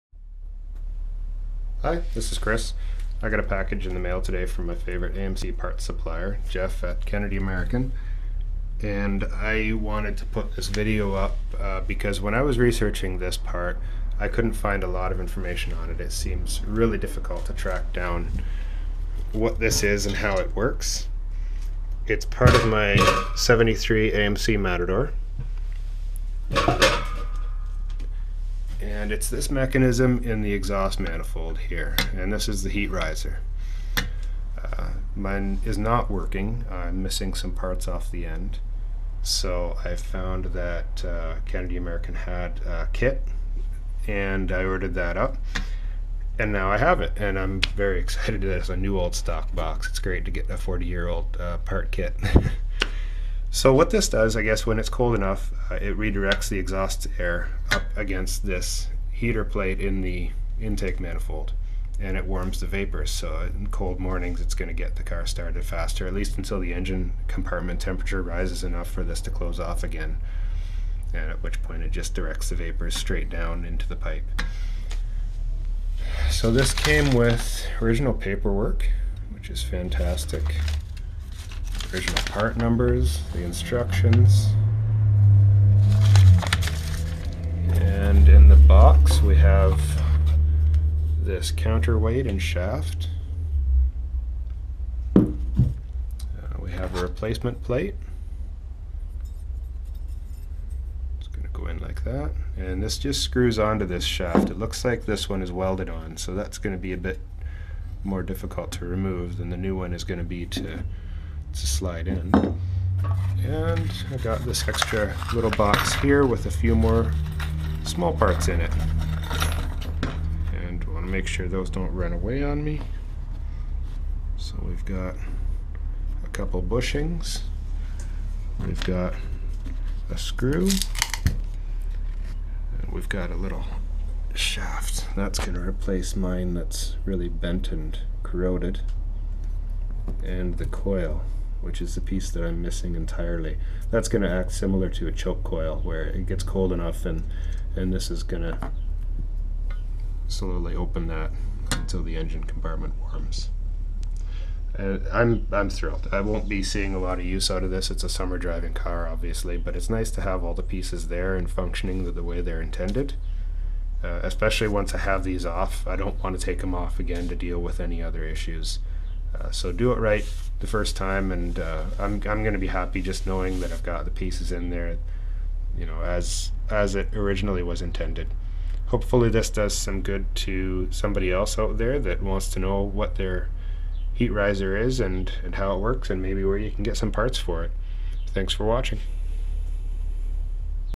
AMC and Jeep Exhaust Manifold Heat Riser
A quick overview of what a AMC (jeep) heat riser looks like and does in a 232 manifold.  Shown with replacement parts to repair it.

Video is based on the manifolds from a 1973 AMC 232, 6 cylinder.

for the hard to find AMC parts!